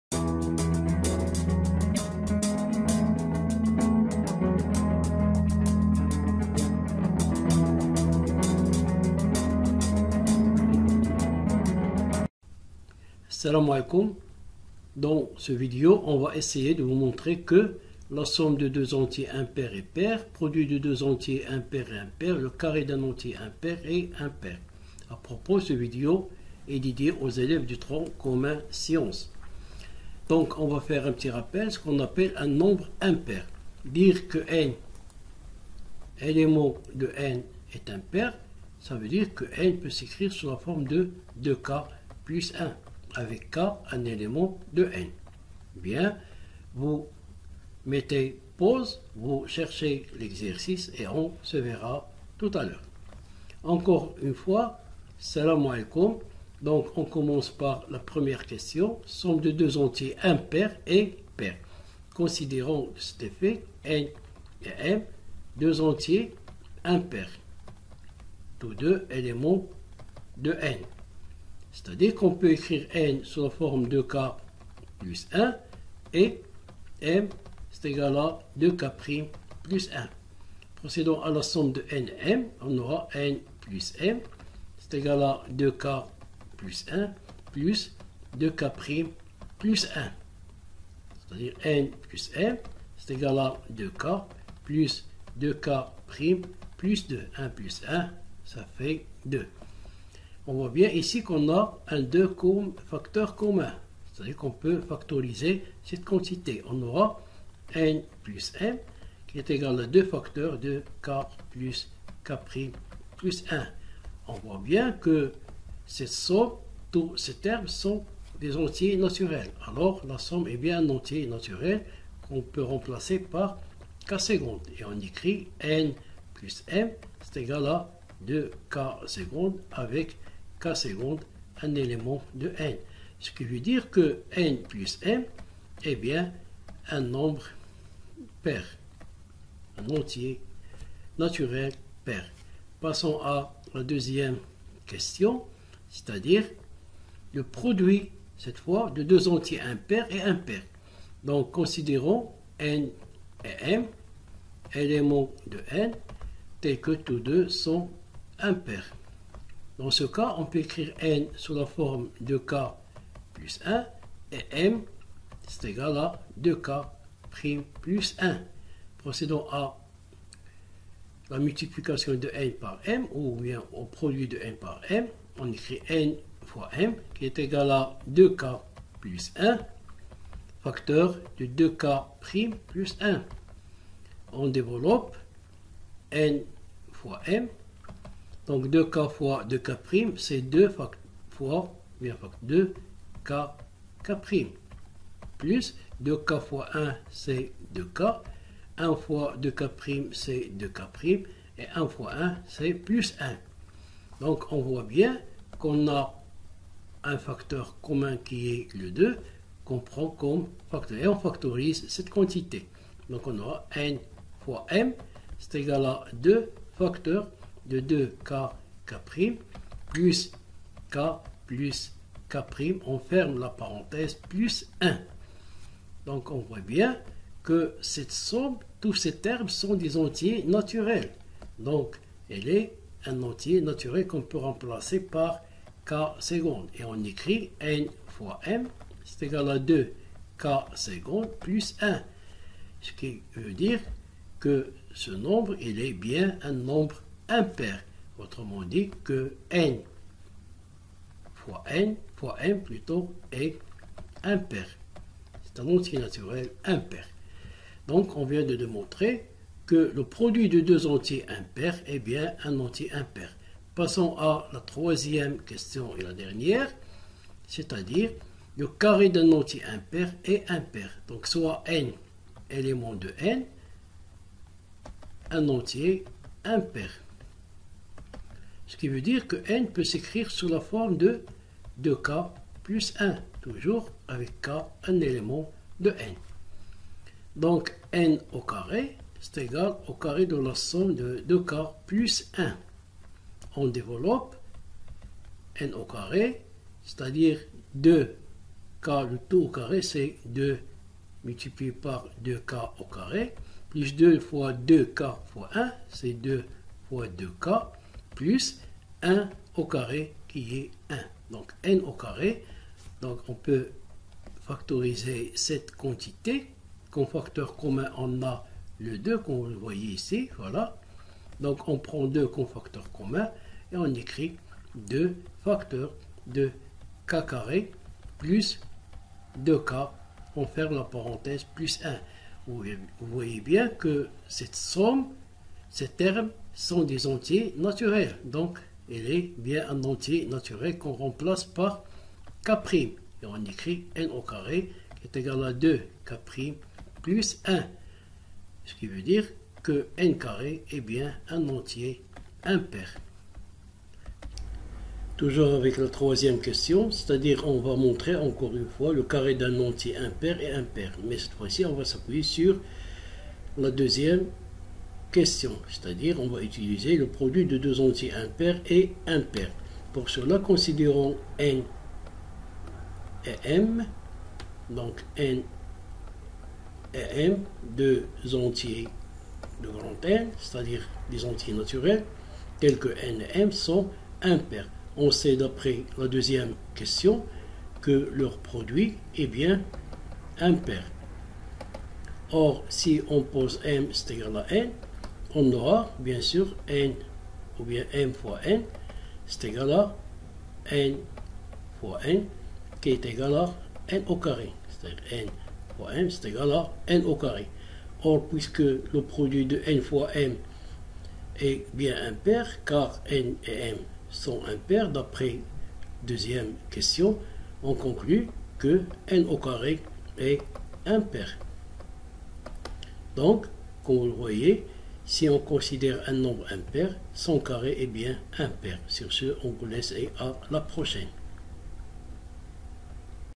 💡 Parité d'un entier naturel Exé n°3
Parité d'un entier naturel.
1) Somme de deux entier impairs est pair.
2) Produit de deux entiers impairs est impair.
3) Le carré d’un entier impair est impair.